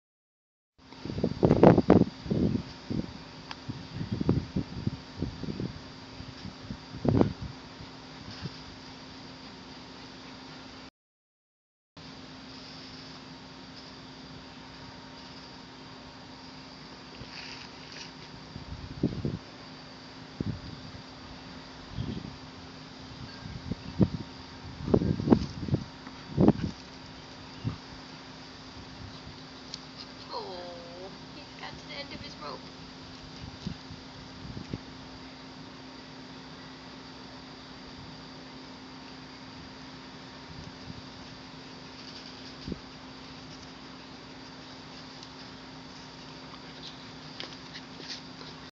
Stoop Doggy Dog
Hanging out with Toby the three month old bernese mountain dog on the stoop.